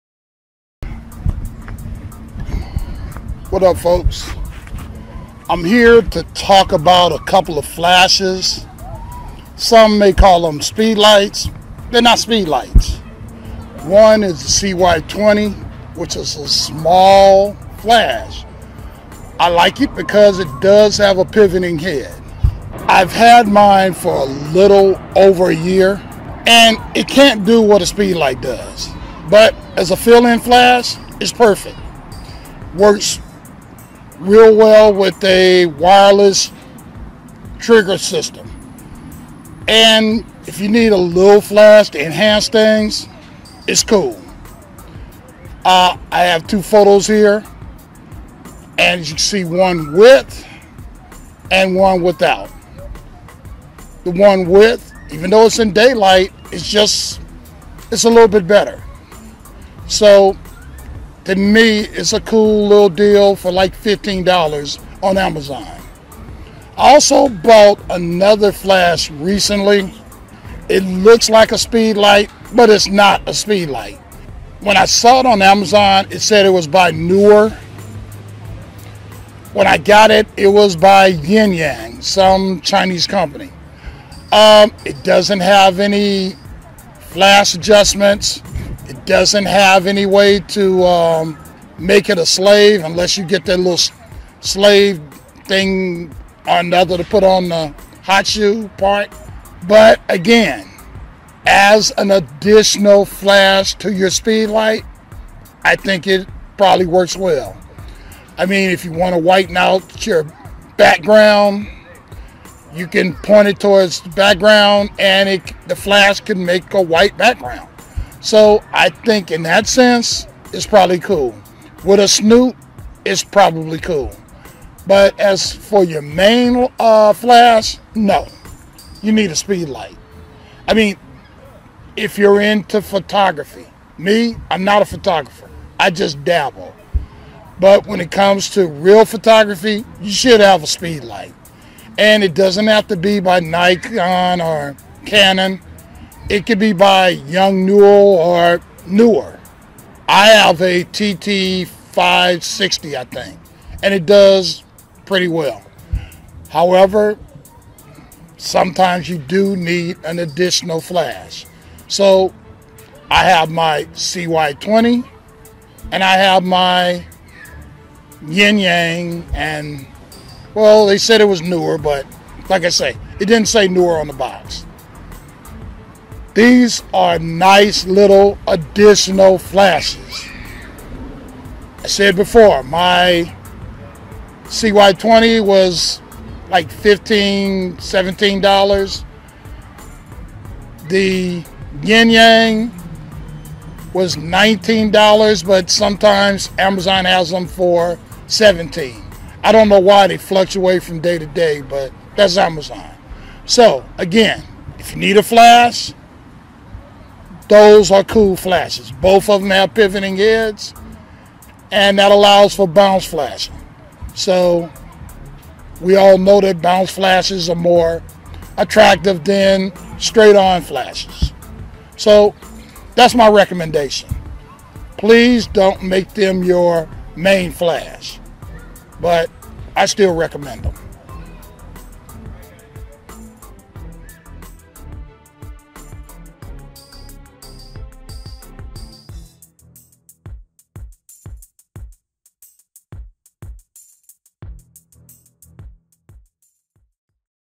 MY 2 EXTRA FLASHES
Neither is a real speedlite (speedlight), but they can enhance and work out in a pinch